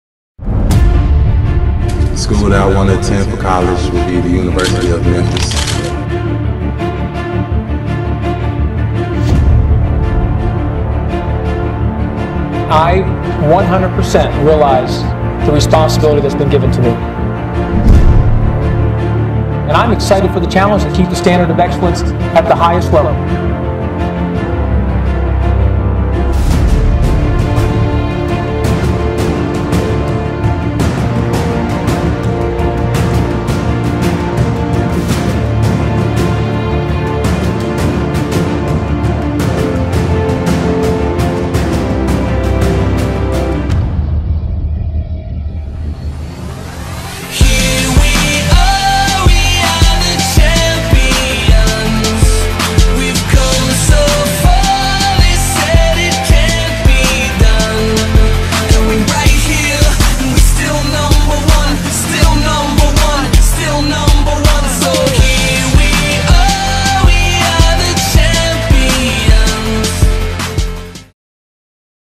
Memphis Tigers - The Journey Continues (2012-13)
Hype video for the Memphis Tigers 2012-13 basketball season. Hope you enjoy!